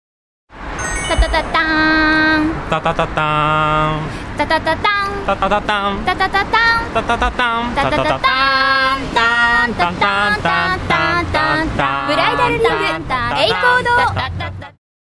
テレビでプロポーズ
今田陸夫さん・友子さんのテレビでプロポーズ。岡山県児島で婚約指輪（エンゲージリング）・結婚指輪（ マリッジリング）・ジュエリー・メガネ・アイウェア・ウォッチ・時計を扱うエイコー堂の提供です。